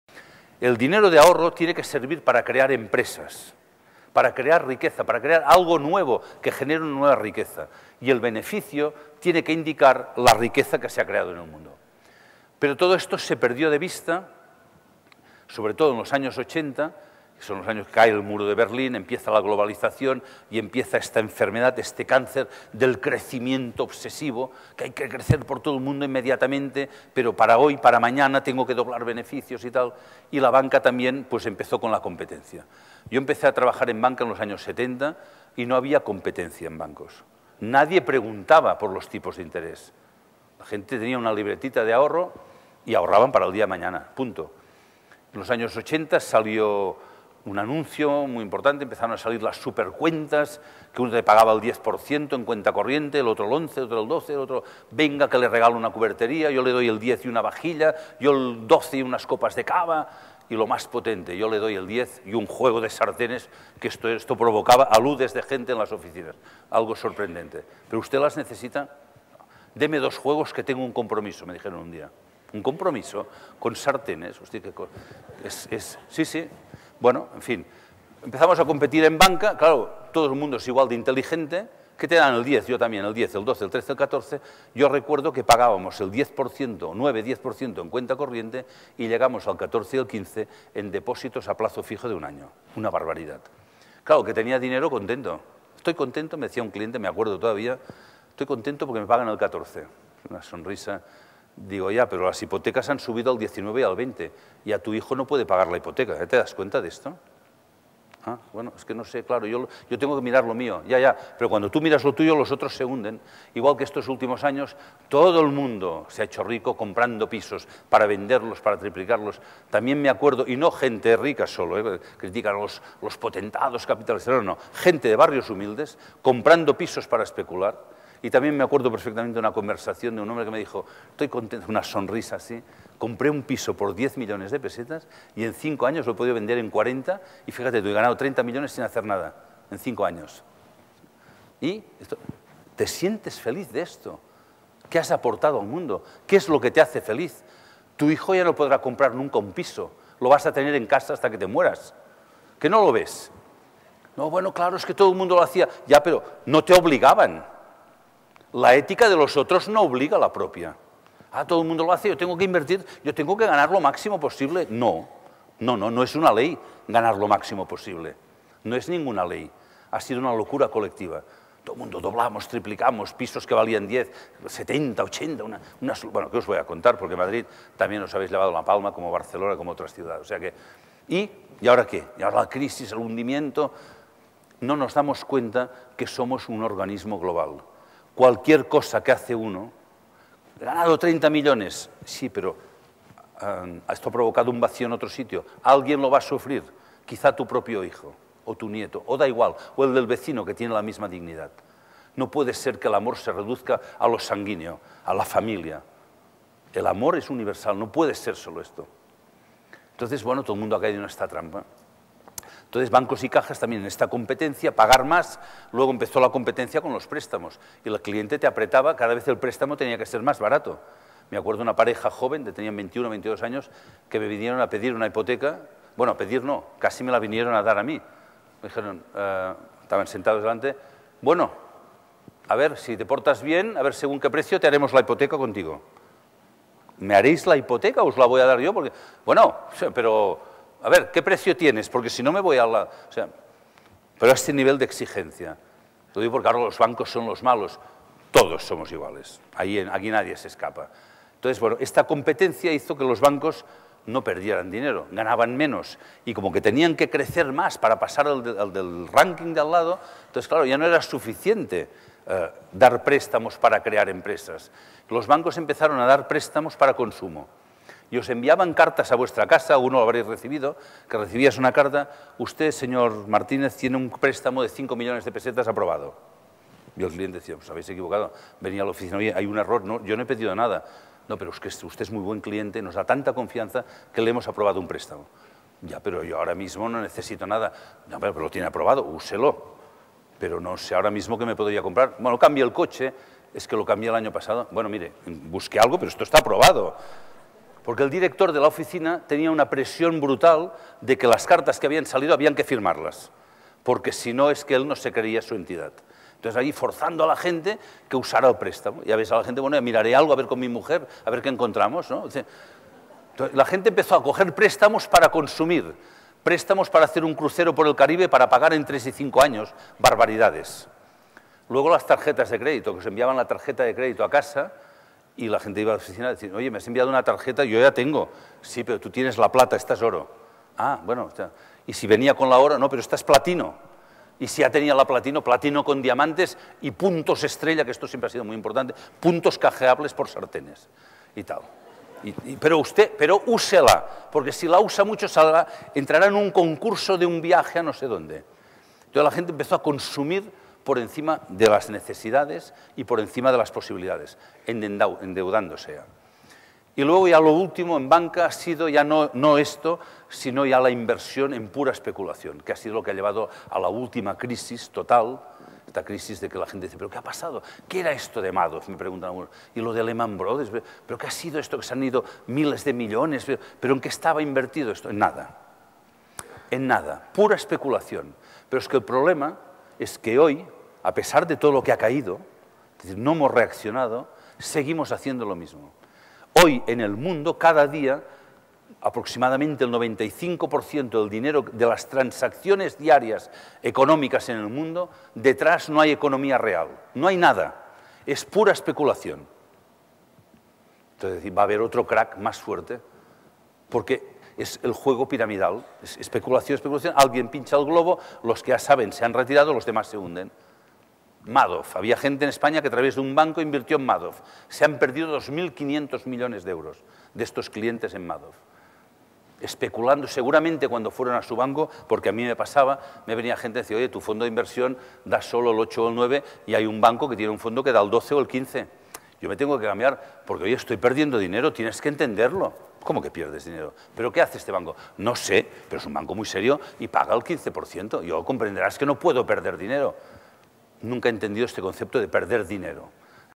Conferencia Joan Antoni Melé 10/17
Disfruta de la conferencia "Dinero y conciencia; ¿a quién sirve tu dinero?" de Joan Antoni Melé para Gente Imprescindible de Mediapost.
(Décima parte)